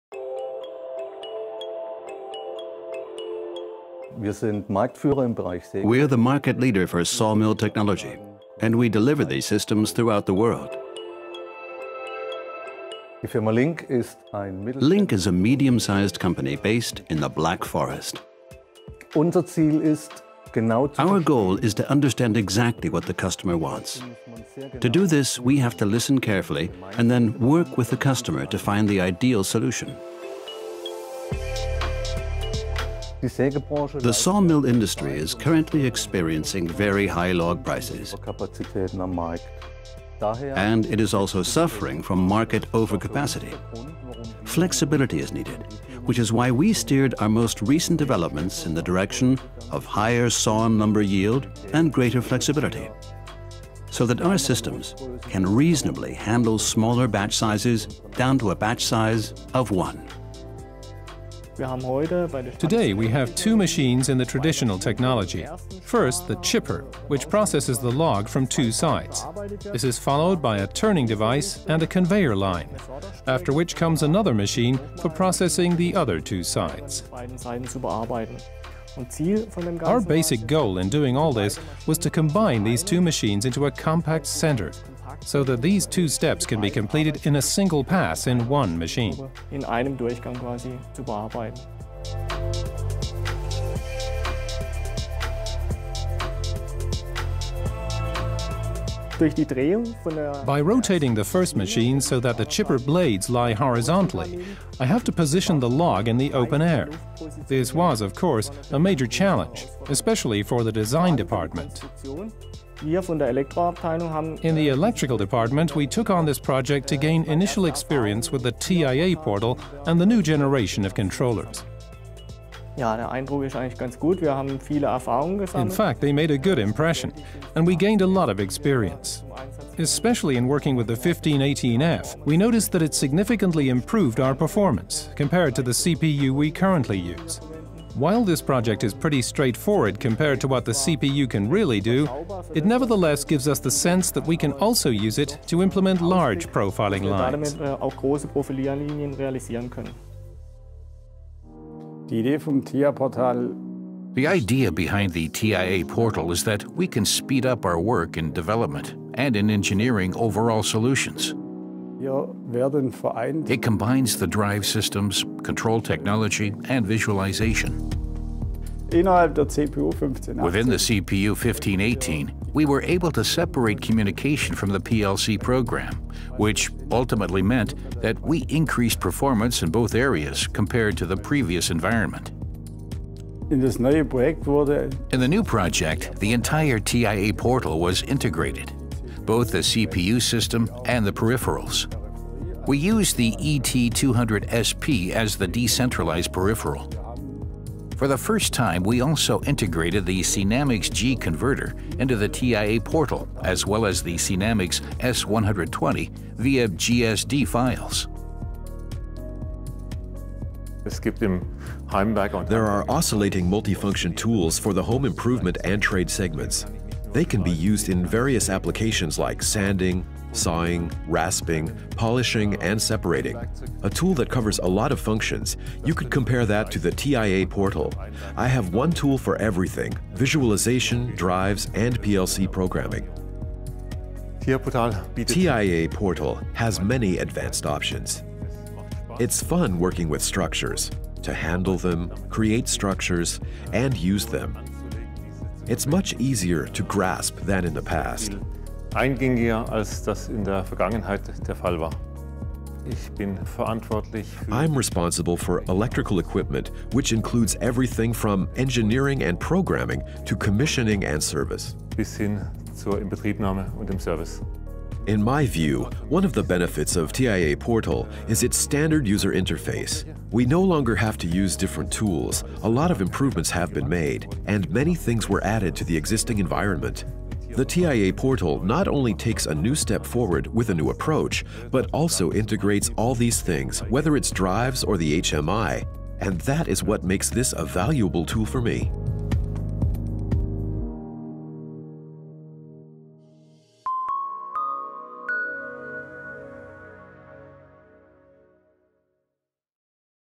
A new compact four-edge chipping machine developed by LINCK
Linck Holzverarbeitungstechnik latest development is a compact four-edge chipping machine for processing small-diameter timber, which can be retrofitted even in existing plants with little space. Equipped with modern control technology from Siemens, the machine ensures continuous log guidance for maximum yield and high scantling quality.